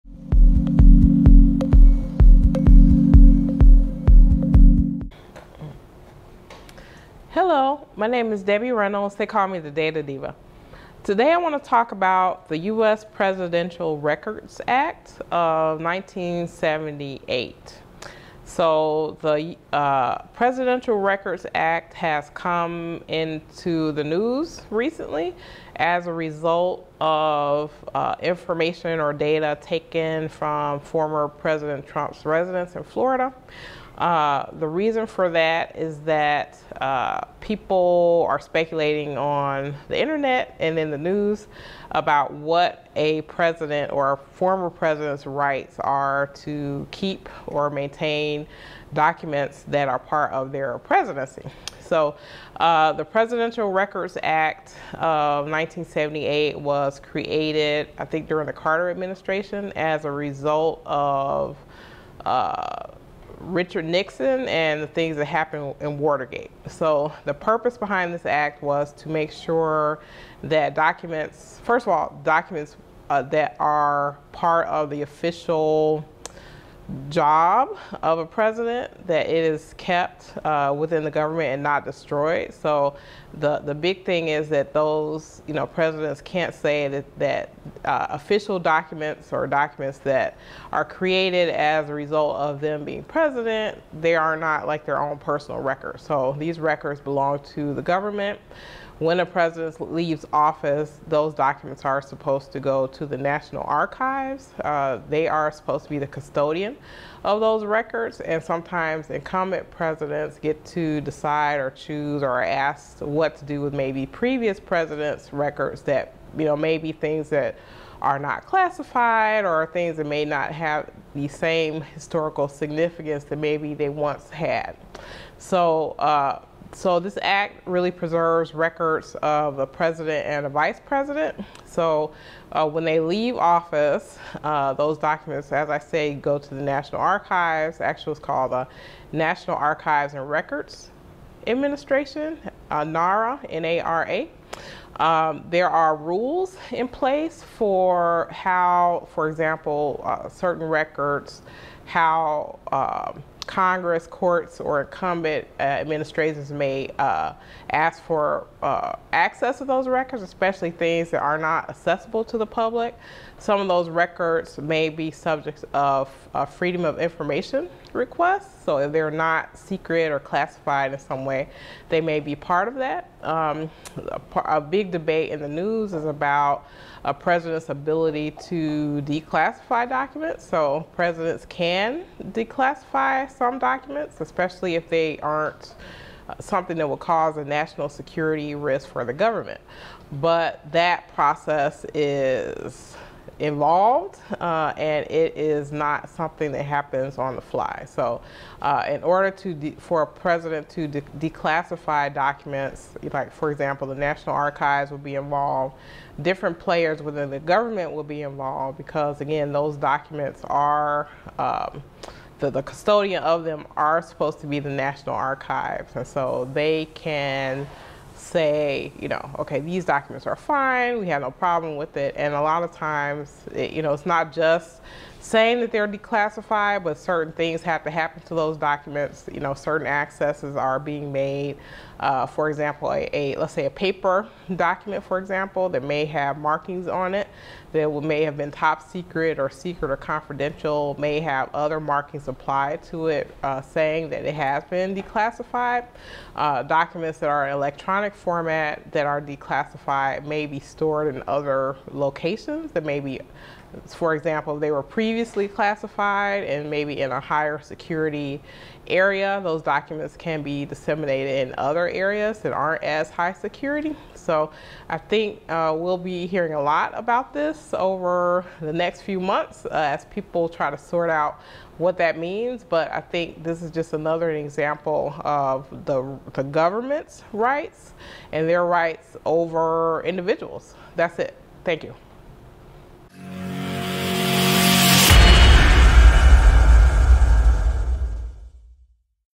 Debbie Reynolds the "Data Diva" Discusses Data Privacy And The Presidential Records Act Of 1978
Did you know that the Presidential Records Act of 1978 (PRA) was created to establish the government as the owner and custodian of Presidential records of Presidents? As a result of the recent FBI raid on former President Trump’s residence to retrieve classified documents, there has been a renewed interest in understanding how Presidential records are handled. Debbie Reynolds, “The Data Diva”, discusses the genesis of the Presidental Records Act of 1978 as a response to Watergate and to ensure the preservation of Presidential records, establishing the National Archives and Records Administration (NARA) as the custodian of Presidental records, the rights of incumbent Presidents with former Presidential records, the process for Congress, courts, and government agencies to access classified Presidential records, Freedom of Information Act (FOIA) requests for unclassified Presidential records, the ability, and process for sitting presidents to declassify documents, declassification of electronic or paper documents, the national security considerations for declassification of documents, the government’s rights over an individual’s rights to Presidential records and Data Privacy in five minutes. Data Privacy and cybersecurity experts, please give me your thoughts. Debbie Reynolds Consulting, LLC Data Diva Media